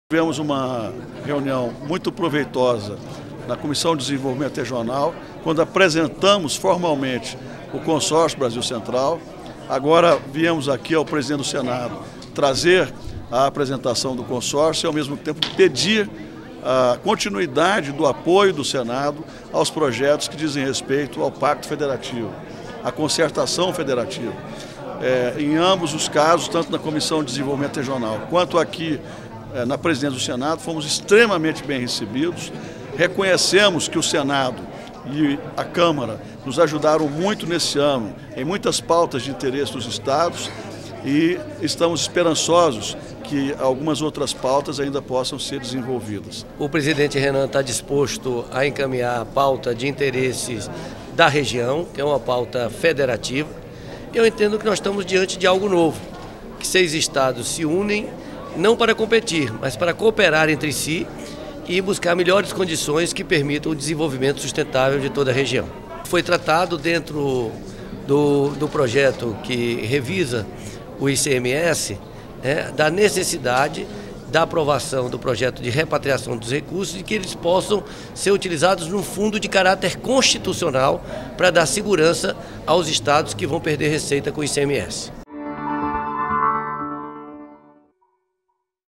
Governadores pedem apoio ao Pacto Federativo
Em encontro com o presidente do Senado, Renan Calheiros, nesta quarta-feira (11), os governadores Rodrigo Rollemberg (Distrito Federal) e Marconi Perillo (Goiás) apresentaram as propostas do Consórcio Brasil Central, que reúne os estados da Região Centro-Oeste, além de Tocantins e Rondônia. Uma das propostas é o uso da repatriação de recursos depositados ilegalmente no exterior, tema do PLS 298/2015, em um fundo de compensação dos estados que sofram perda de receita com a reforma do ICMS.  Os governadores reiteraram a Renan o apoio ao Pacto Federativo, para corrigir desigualdades na relação entre União, estados e municípios.